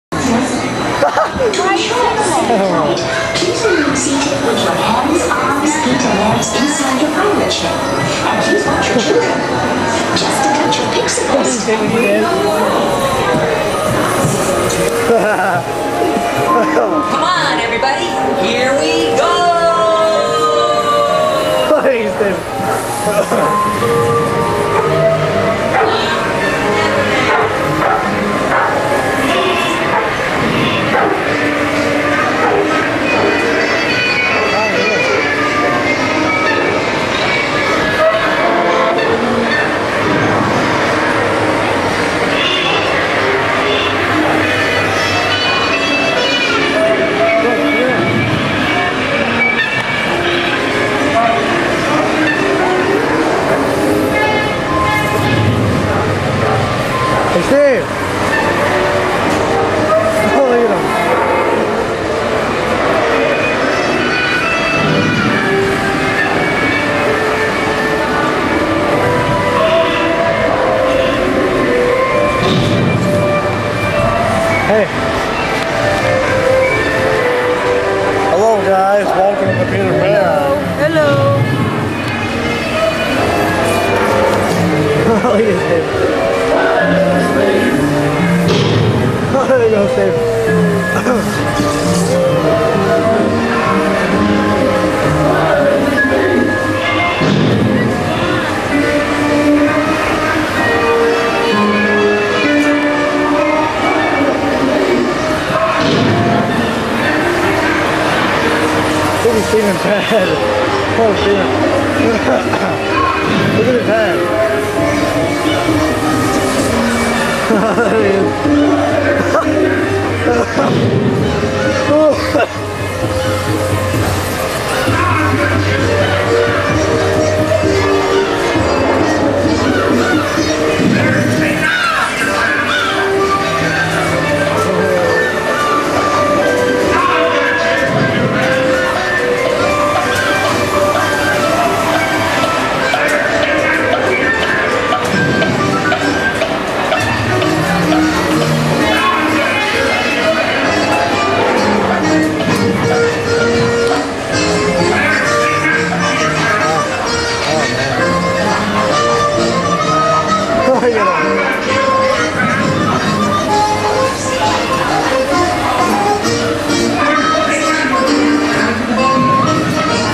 PETER PAN'S FLIGHT AT WALT DISNEY WORLD 2009
COMPLETE DARK POV OF PETER PAN'S FLIGHT AT WALT DISNEY WORLD